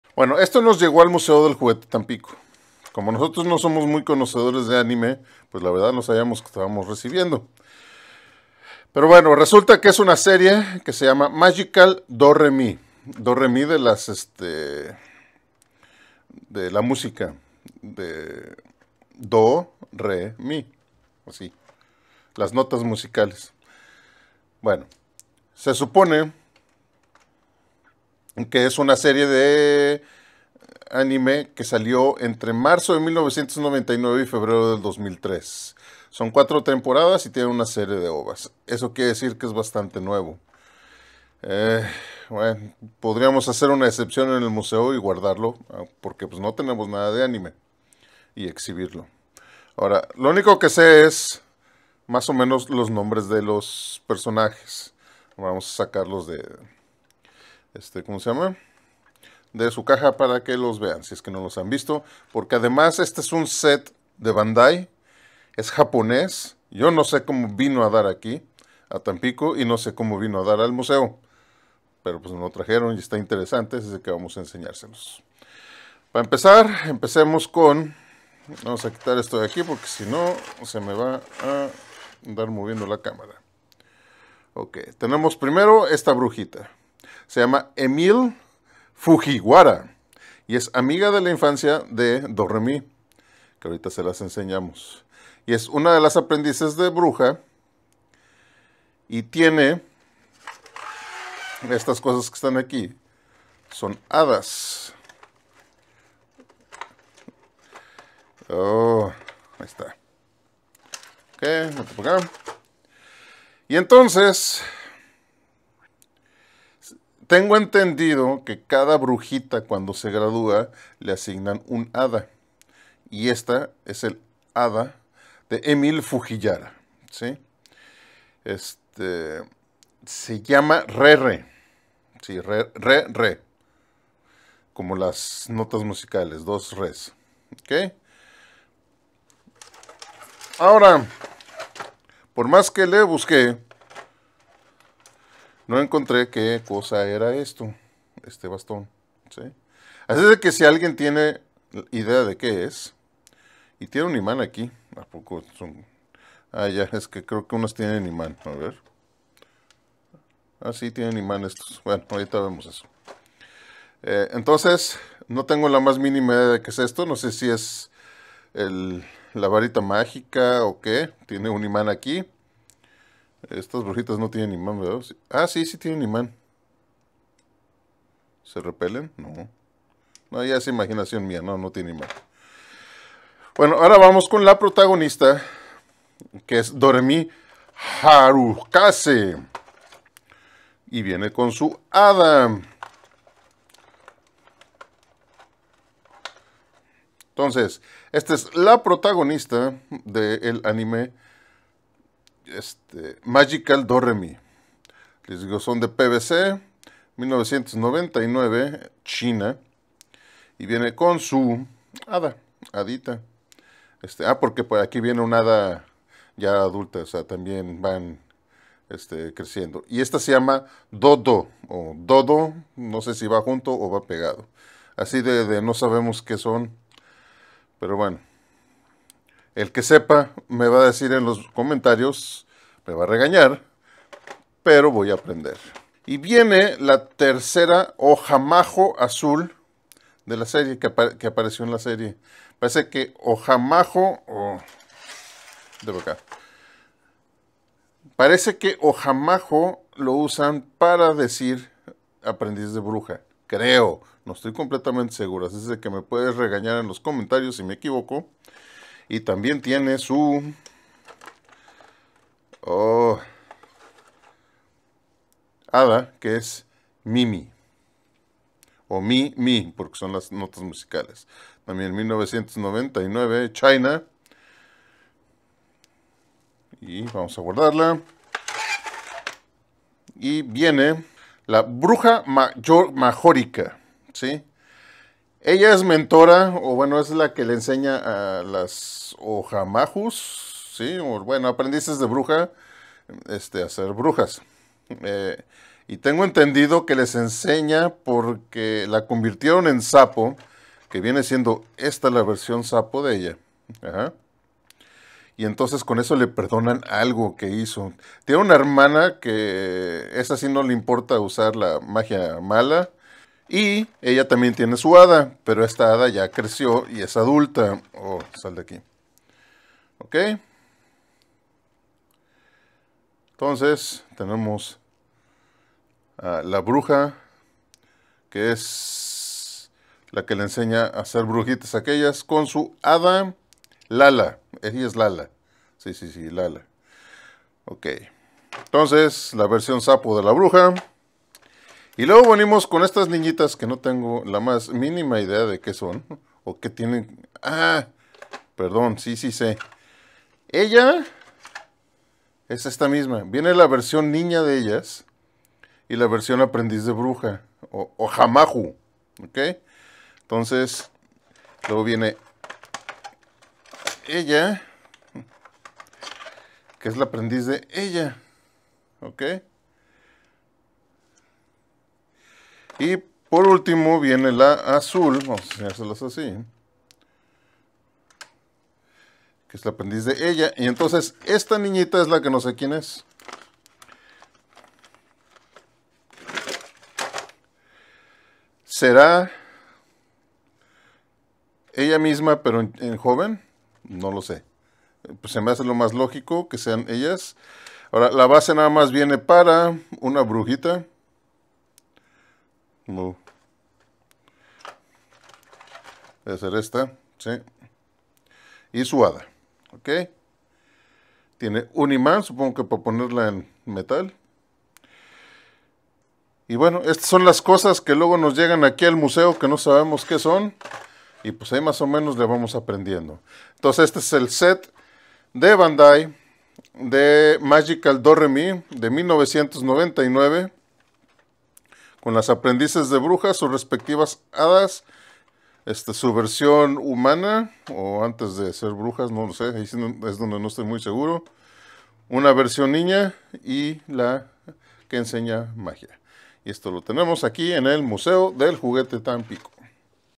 Magical Doremi 1999 Bandai - fojamajo doremi
Pues nos llego este set de figuras bandai de Magical doremi no lo conociamos porque no sabemos mucho de anime para el video archivo del @museodeljuguetetampico